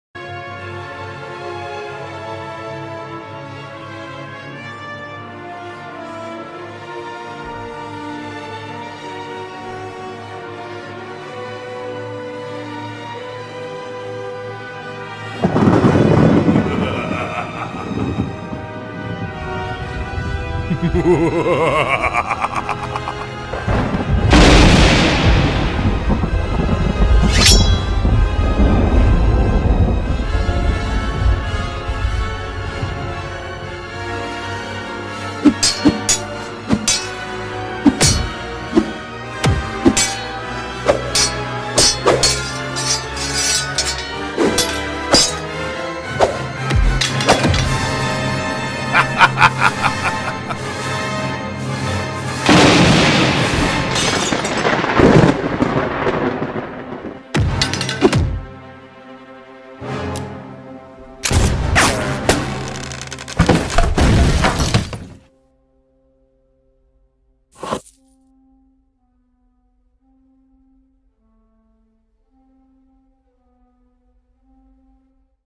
Johnny Thunder Vs Sam Sinister | LEGO Sword Duel
A sword duel between Johnny Thunder and Sam Sinister, made for both the weekly BiM Animation challenge and Brickfilms.com's Silent contest.